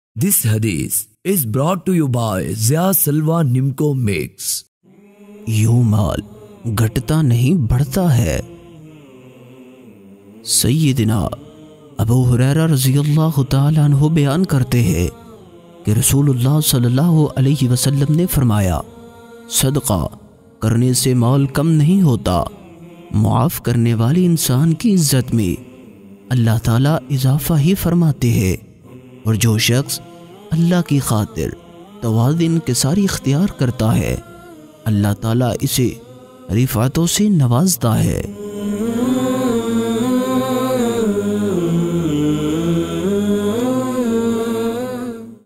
Nabi SAW ne farmaya Sadqa kro Sadqa karne se mal kam nahi hota | Hadith Mubarak | Zia Salva Nimko
Zia Foods has started a series of 'Hadith' on YouTube channel, that will help in enhancing islamic knowledge in youth and adults. May Allah SWT shower His countless blessings on us.
Aameen